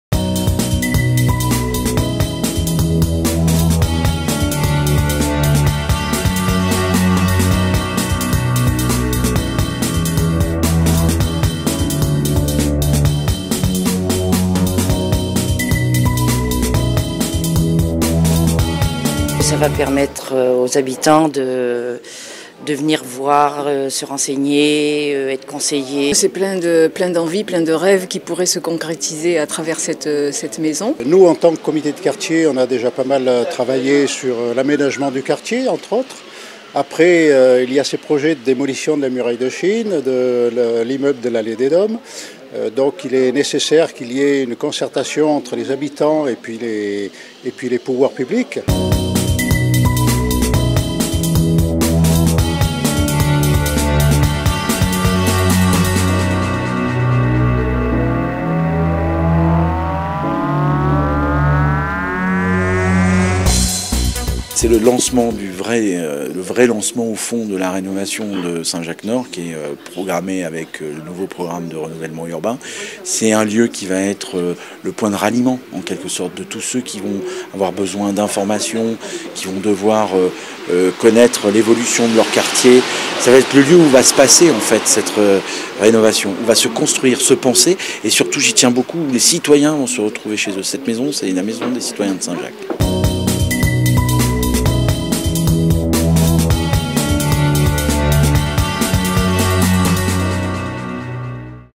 Renouvellement urbain : Inauguration de la maison du projet à St-Jacques Nord
Dans le cadre du Nouveau Programme de Renouvellement Urbain (NPRU), Olivier Bianchi, Président de Clermont Auvergne Métropole, a inauguré, le 30 septembre dernier, la Maison du projet de Saint-Jacques Nord. Un lieu d’information, d’animation et de concertation, ouvert à tous les habitants du quartier.